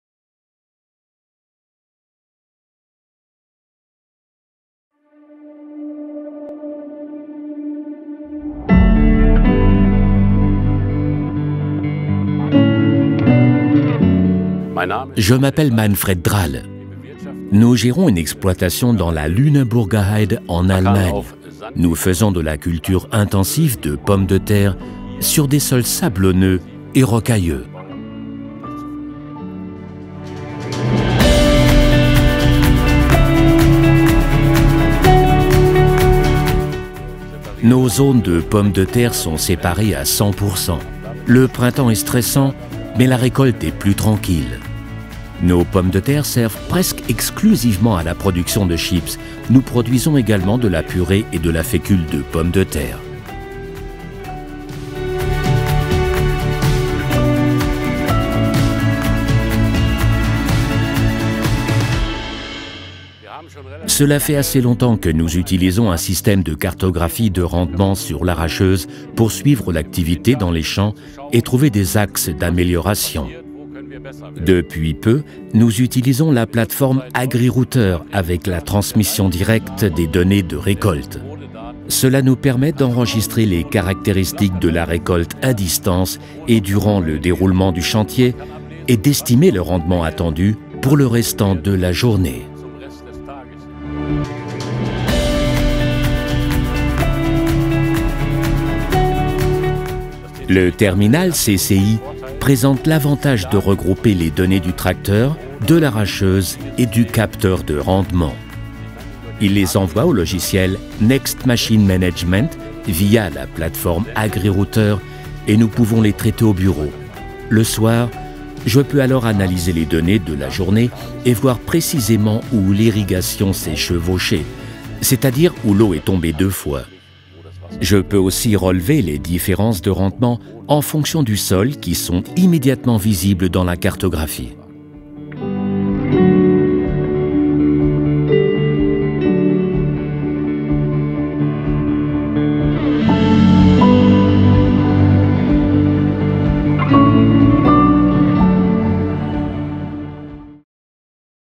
Récolte de pommes de terre avec transmission en direct des données de rendement
Récolte de pommes de terre avec transmission en direct des données de rendement / agrirouter Data Exchange